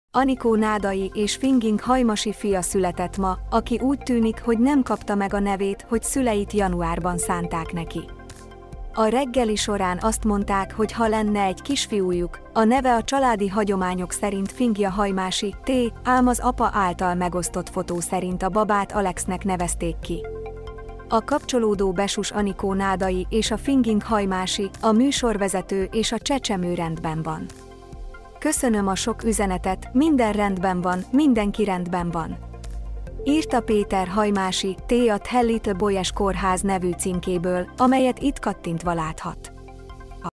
Ennek ellenére Péter Hajmássy Pál lett Anikó Nádai kisfiúja
Alexnek nevezik.
Anikó Nádai és Finging Hajmasy fia született ma, aki úgy tűnik, hogy nem kapta meg a nevét, hogy szüleit januárban szánták neki. A reggeli során azt mondták, hogy ha lenne egy kisfiújuk, a neve a családi hagyományok szerint fingja Hajmássy -t, ám az apa által megosztott fotó szerint a babát Alexnek nevezték ki. A kapcsolódó Besus Anikó Nádai és a Finging Hajmásy, a műsorvezető és a csecsemő rendben van. Köszönöm a sok üzenetet, minden rendben van, mindenki rendben van! - Írta Péter Hajmásy -t a The Little Boy's Kórház nevű címkéből, amelyet itt kattintva láthat.